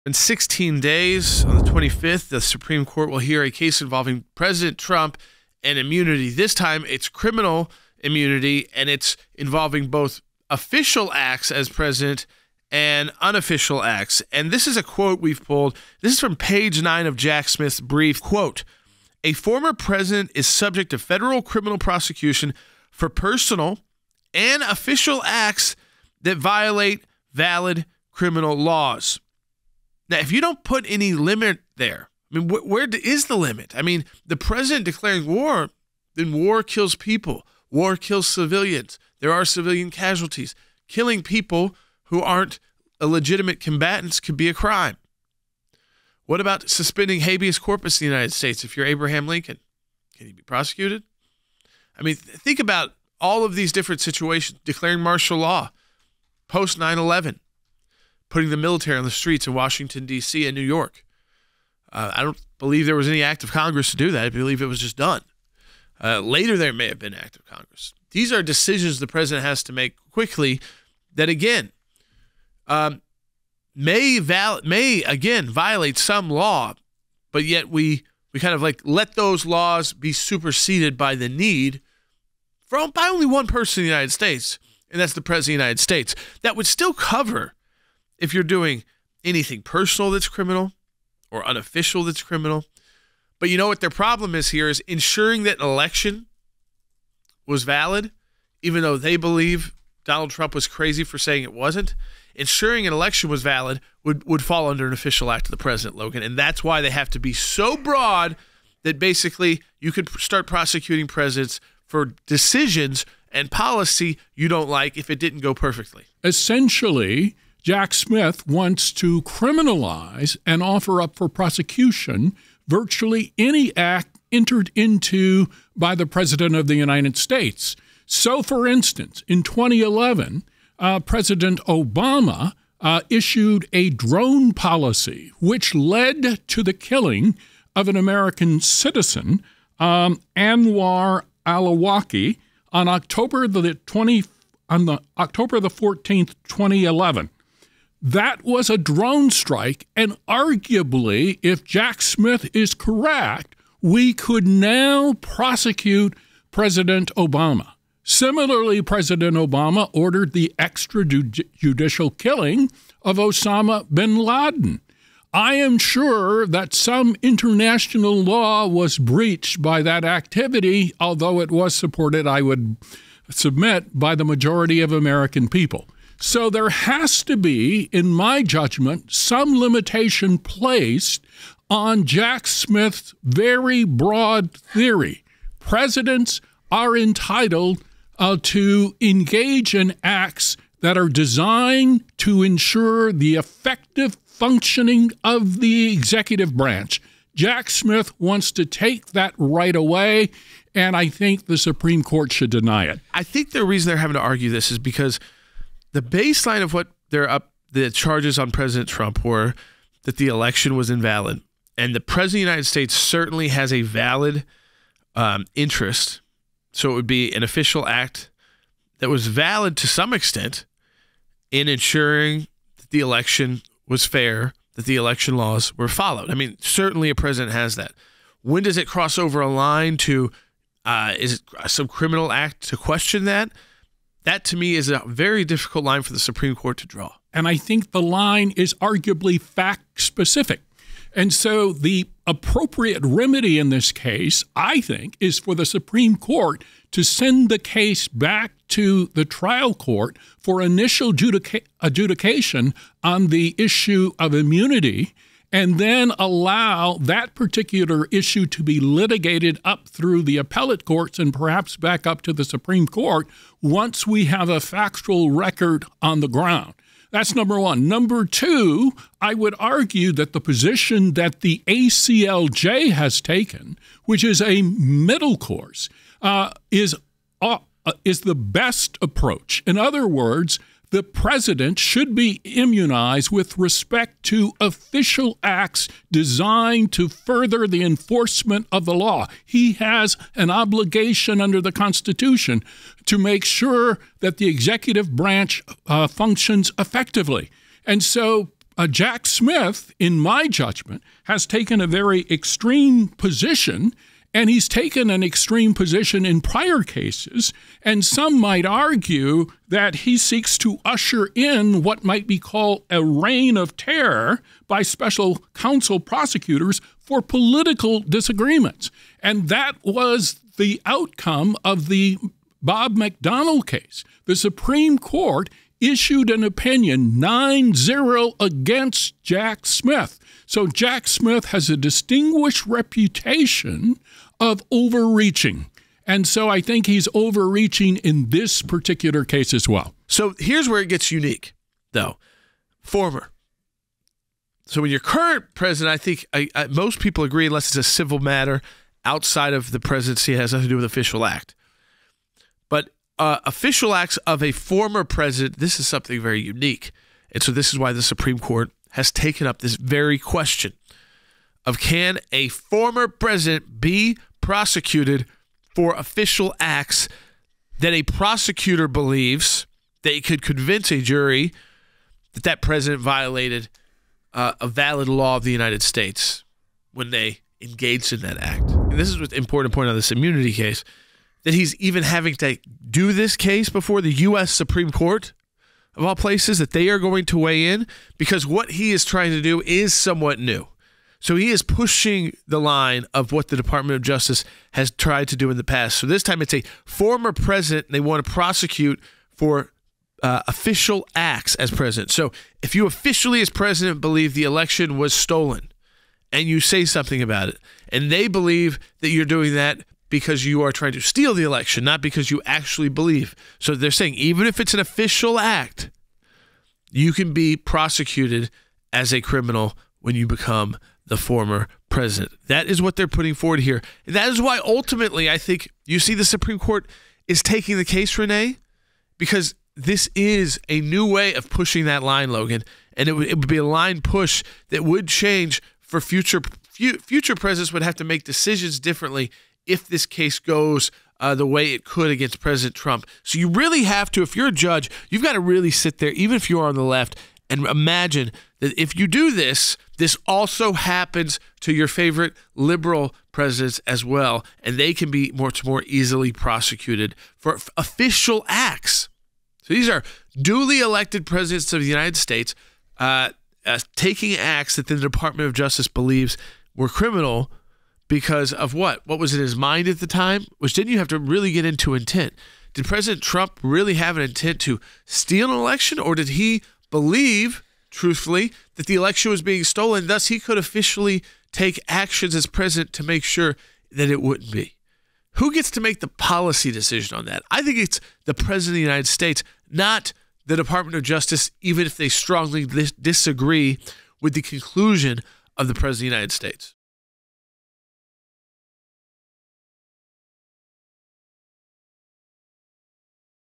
Jack Smith Is Melting DOWN Over TRUMP’S Immunity
Special Counsel Jack Smith just urged the U.S. Supreme Court to reject President Donald Trump’s claim to presidential immunity. Smith has charged Trump with conspiracy to overturn the results of the 2020 election against President Joe Biden. Will the Justices heed Smith’s request?